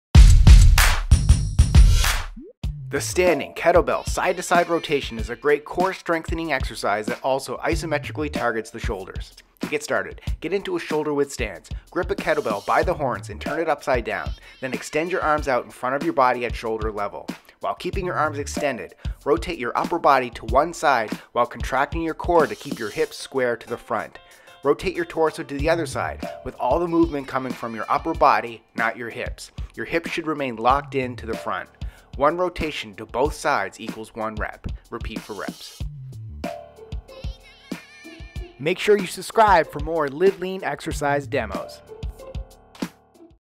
How To: Standing Kettlebell Side To Side Torso Rotation
Let's fire up those core muscles and shoulders with the Standing Kettlebell Ball Side To Side Torso Rotation. This move hits your core hard while keeping your shoulders working too.

Here’s how to do it properly:
1. Start by standing shoulder-width apart, gripping the kettlebell by the horns, flipped upside down.
2. Extend your arms straight out in front at shoulder height.
3. With your arms locked out, rotate your upper body to one side, engaging your core to keep your hips locked and square to the front.
4. Rotate your torso to the other side, but remember, it’s all about your upper body rotation.
5. Keep your hips locked in place.
6. One full twist to both sides counts as one rep.
7. Repeat for reps.

Standing Kettlebell Side To Side Torso Rotation:
► MUSCLES WORKED: Abs, Core, Obliques
► TYPE: Strength
► EQUIPMENT: Kettlebell
► EXPERIENCE: Intermediate